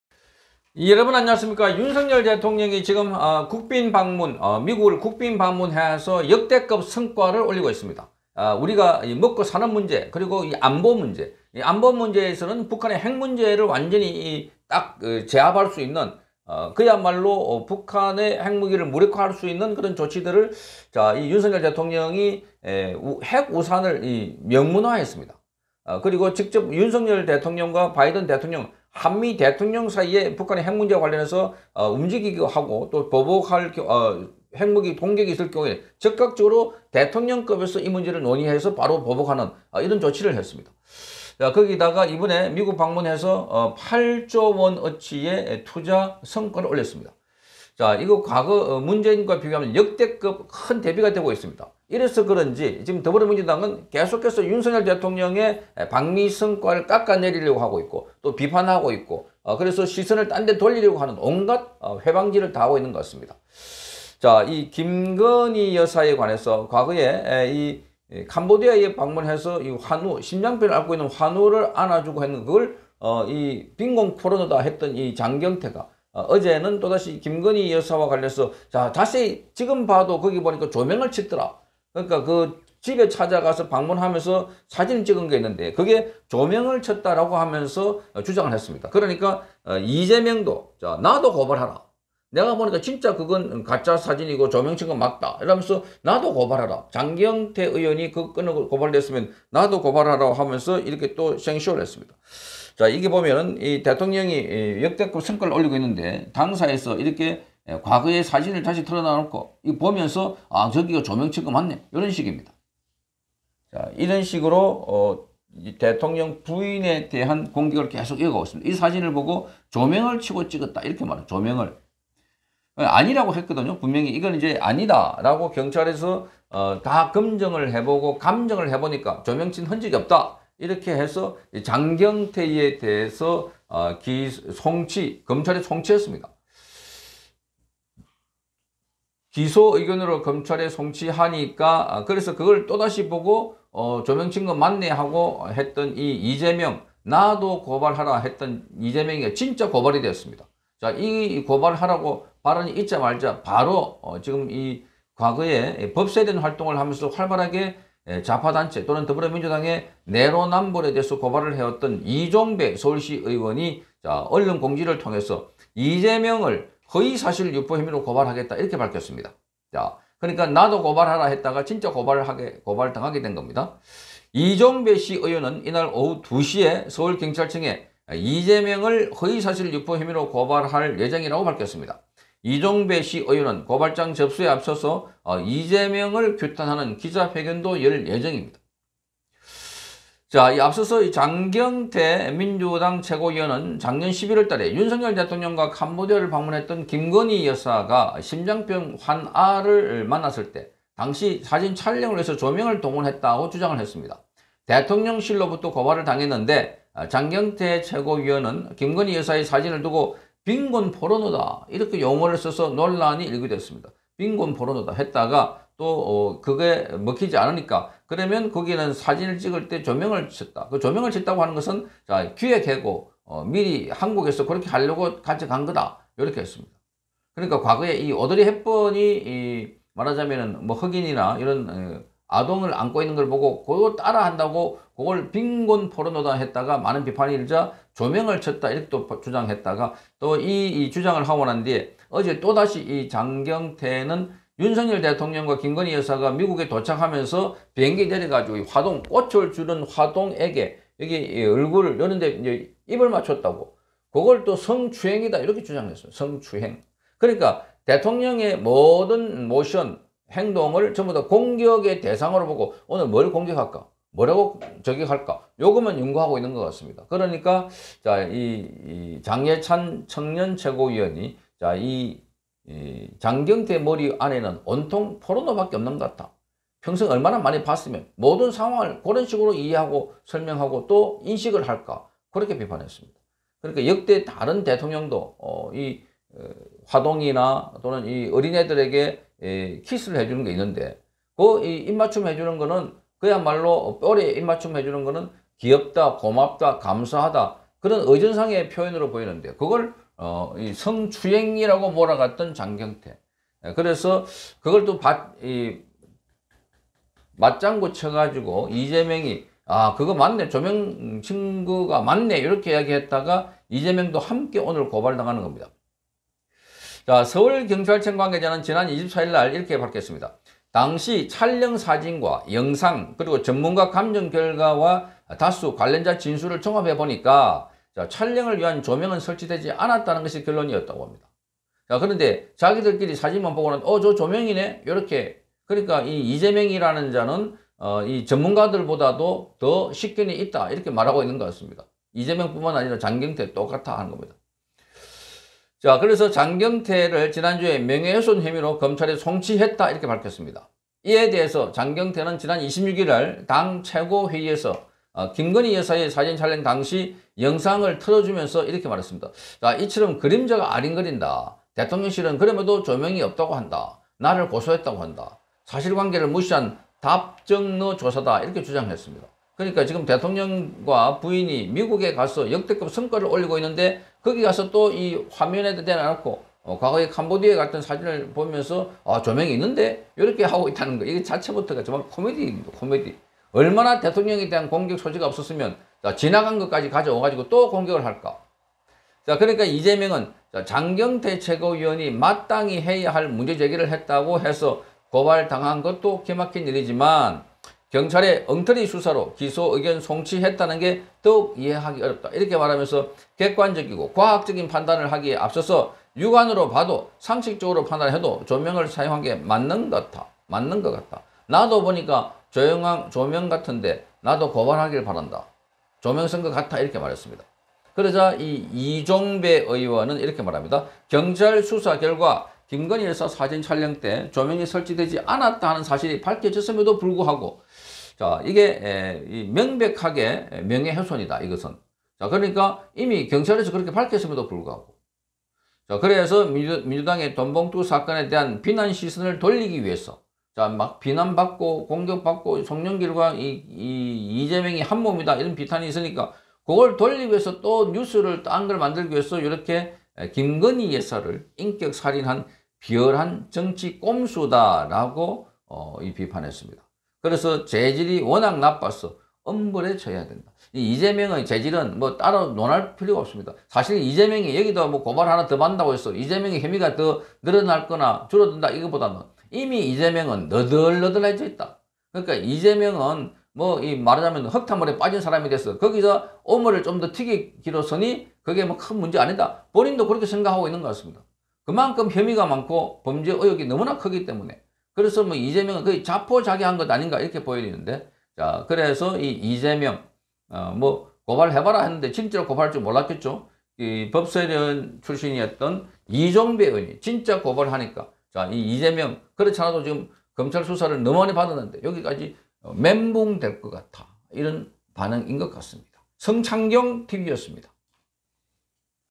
방미 성과내고있는 윤부부 공세...가짜로 판명난 김건희 조명 또 거론하다 진짜 고발당한 이재명
“김건희 조명, 나도 고발하라”던 이재명, 진짜 고발당한다